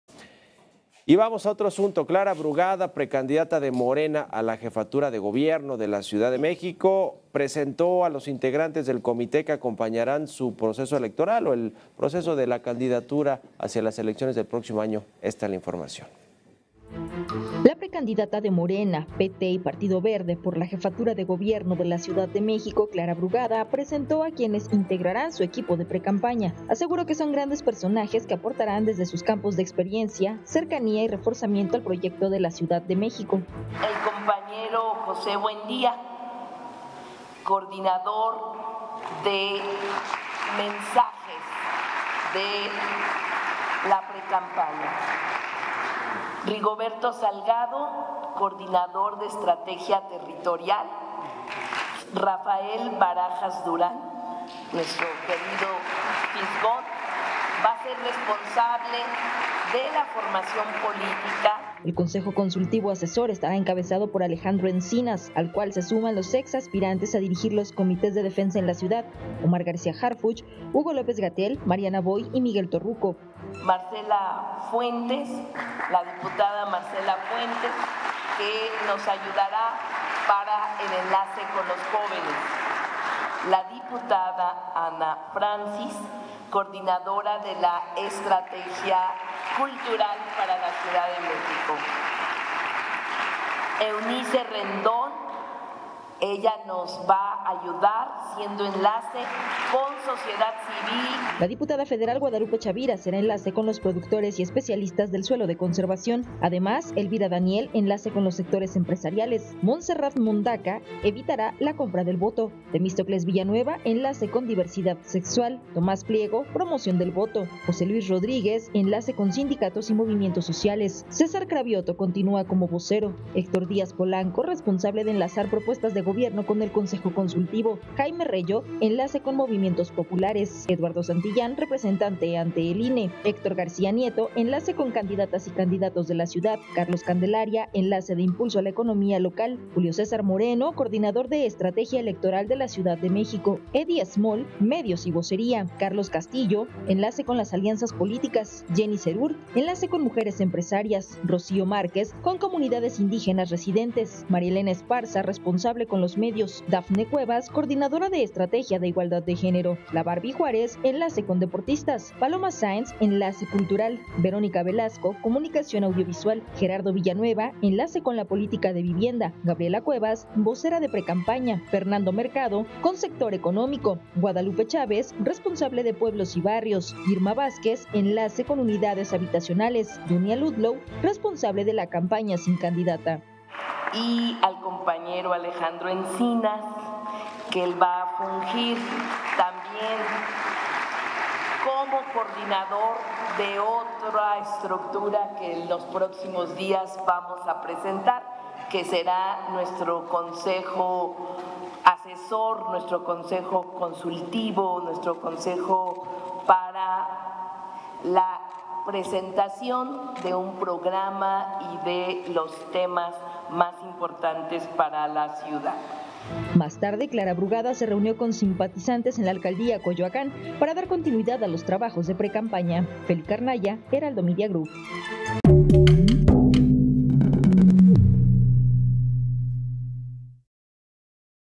De Edy Smol a la “Barbie” Juárez: Clara Brugada presenta a su equipo de campaña rumbo a la CDMX
Este domingo, Clara Brugada Molina, precandidata única de Morena por la jefatura de Gobierno de la Ciudad de México, dio a conocer quiénes conformarán su equipo de precampaña. Desde el Club de Periodistas de México, ubicado en la calle Filomeno Mata 8, colonia Centro de la alcaldía Cuauhtémoc, la exalcaldesa presidió el evento.

Alrededor de las 10:00 am empezaron a llegar simpatizantes del partido guinda, entre ellos la ex panista y ex delegada de Miguel Hidalgo, Gabriela Cuevas; la diputada de Morena, Guadalupe Chavira; y el secretario del trabajo, José Luis Rodríguez.